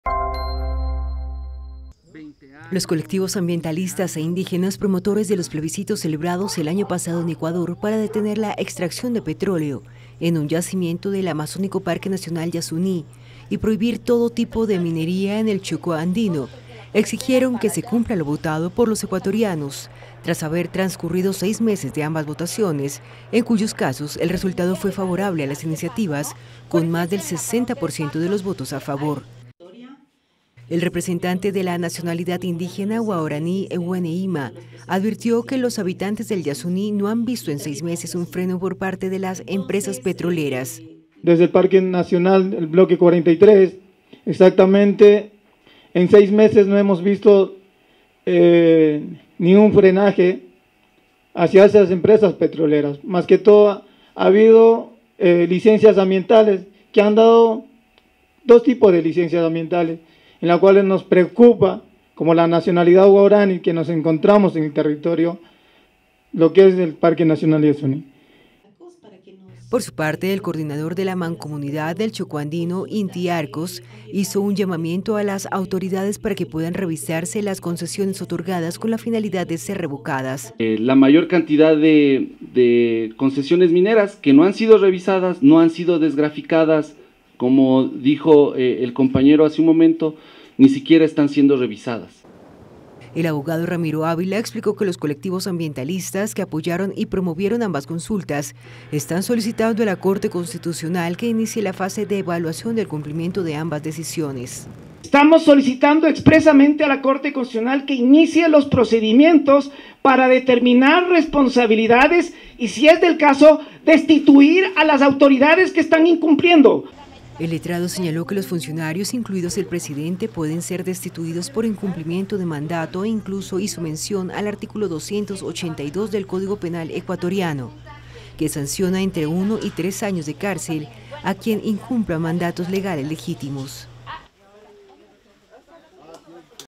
Reclaman cumplimiento de los plebiscitos contra petróleo y minería celebrados en Ecuador
Los colectivos ambientalistas e indígenas promotores de los plebiscitos celebrados el año pasado en Ecuador para detener la extracción de petróleo en un yacimiento del amazónico Parque Nacional Yasuní y prohibir todo tipo de minería en el Chocó Andino exigieron que se cumpla lo votado por los ecuatorianos tras haber transcurrido seis meses de ambas votaciones.
En ambos casos el resultado de las votaciones fue favorable a las iniciativas con más del 60 % de los votos a favor de sendos planteamientos.

📍 Quito, 20 feb
📹 Imágenes: uan Francisco Chávez // Archivo EFE
🎙 Edición y locución: Annia Hidalgo Vargas

¿Te ha gustado este vídeo de la Agencia EFE? Está disponible para descarga en múltiples formatos adaptados a todo tipo de clientes.